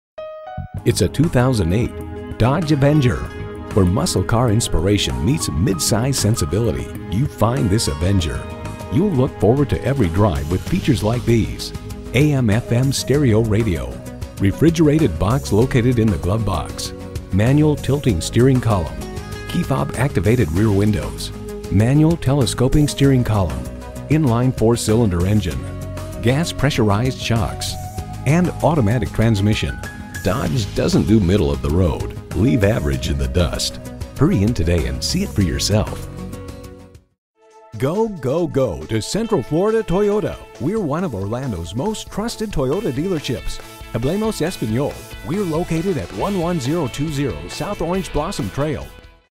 Used 2008 Dodge Avenger Orlando, FL
Year: 2008
Make: Dodge
Model: Avenger
Trim: 4dr Sedan SE FWD
Engine: 2.4L 4 CYLINDER
Transmission: 4-Speed Automatic
Color: Black
Interior: Other
Mileage: 155155
Stock #: 8N240182
VIN: 1B3LC46K78N240182

Address:
11020 S Orange Blossom Trail